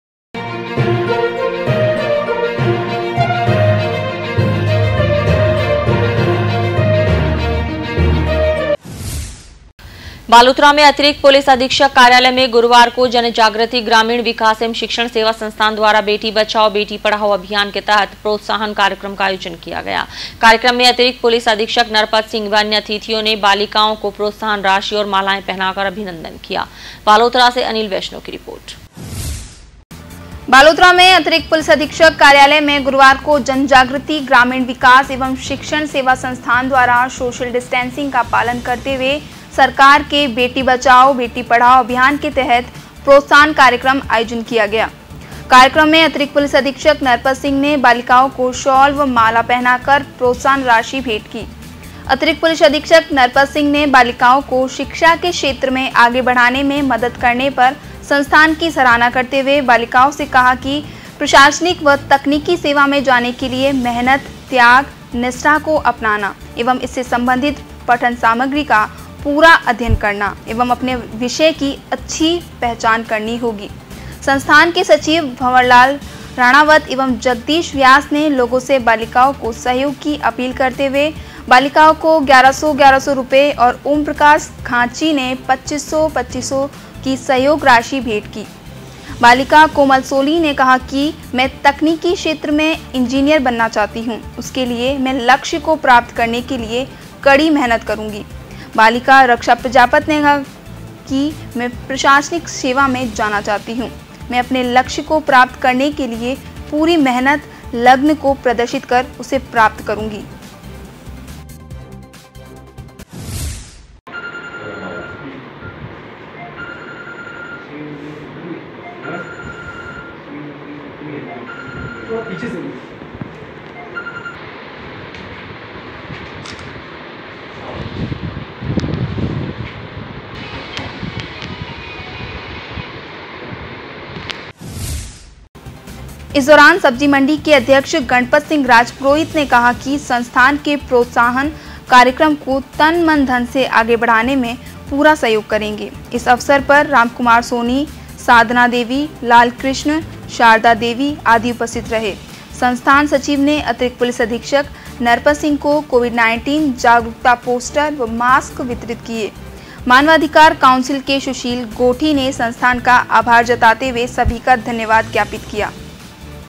जन जागृति ग्रामीण विकास सेवा संस्थान ने प्रोत्साहन कार्यक्रम किया आयोजित, बालिकाओं का हुआ सम्मान
बालोतरा में जन जागृति ग्रामीण विकास सेवा संस्थान ने प्रोत्साहन कार्यक्रम किया आयोजित, बालिकाओं का हुआ सम्मान